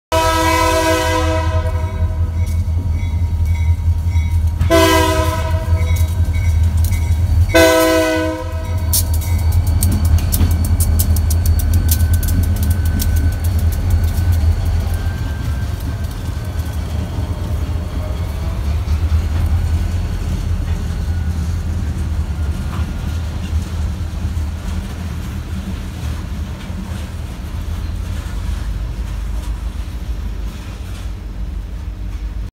AVR-3 with 2 GP40s southbound
AVR-3 heads south toward Washington crossing a private road near The Country Store in Venetia, PA. A shorter train than normal, the power was “Gray Wolf” GP40 4003, and the mother-slug pair of GP40 4005 and slug 405.